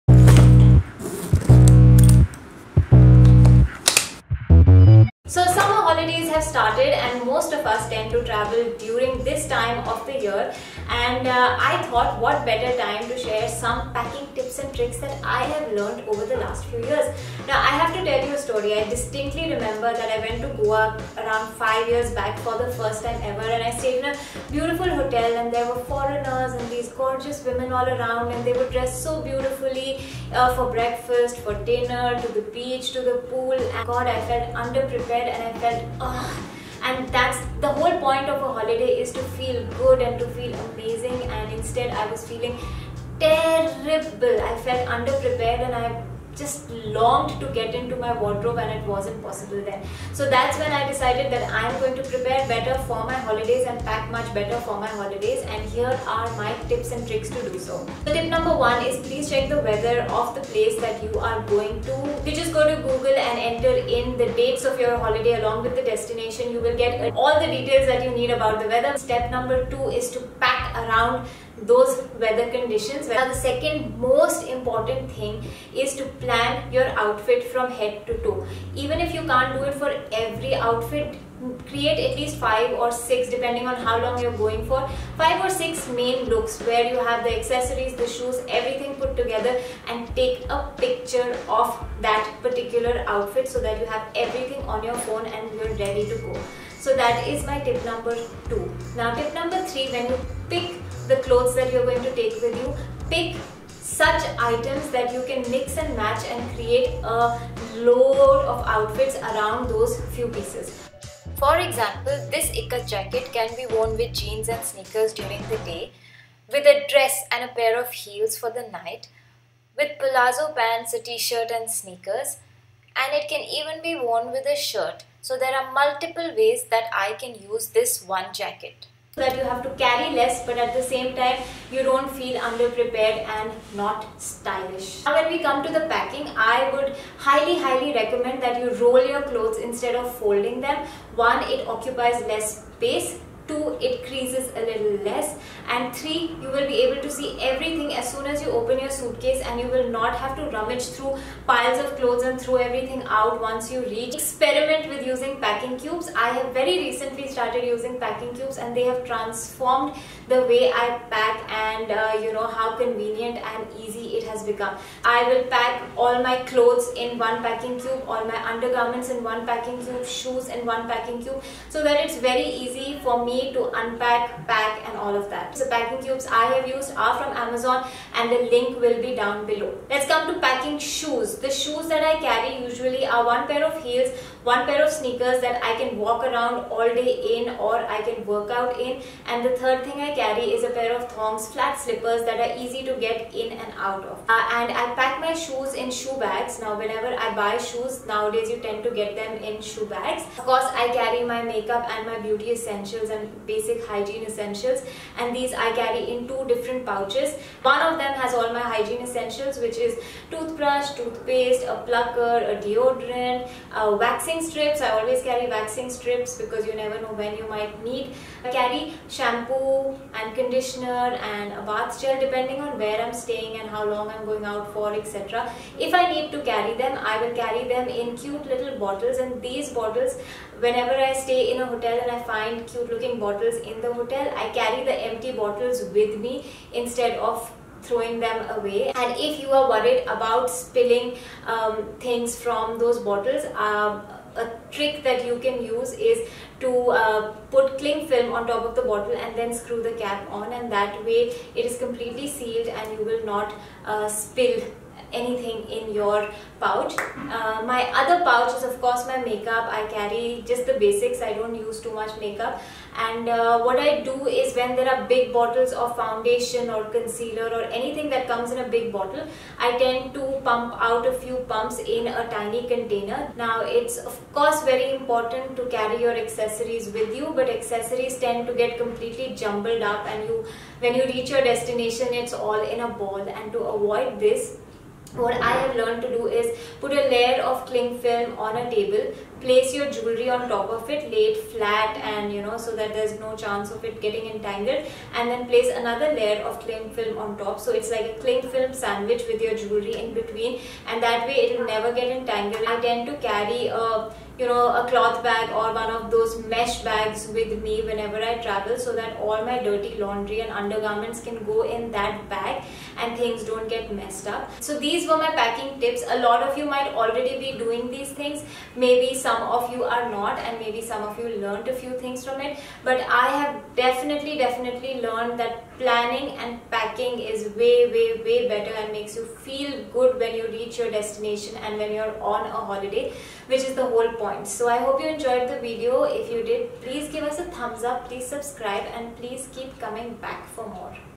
My Packing Tips Tricks & Hacks : What I Learnt About Packing In The Last 5 years
Packing Tips Tricks & Hacks
After I got married and went for my first real holiday I felt underprepared and not pretty. The whole point of a holiday is to relax and feel good, but because I did not plan and pack well, I felt not good.
So I decided to start planning and packing better. I learnt a few packing tips and tricks along the way that help me tremendously when it comes to holiday packing.
Staying organised with your packing is the key to having a fantastic holiday and come back feeling like a million bucks.
I am sure most of you might be following these tips already but maybe you will learn a few new tricks through this packing hacks video.

LINKS
Packing Cubes